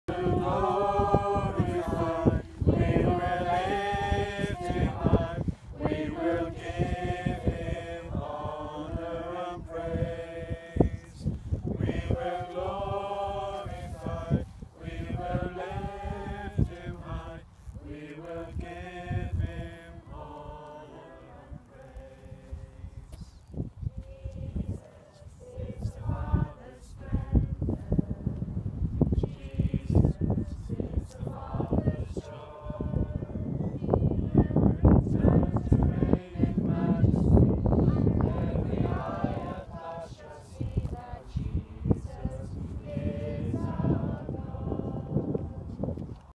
Carrying the Cross at Abergavenny, May 2016
Cross Carrying event in Abergavenny. Morning started with taking the cross to the top of The Blorenge overlooking the town from the south side.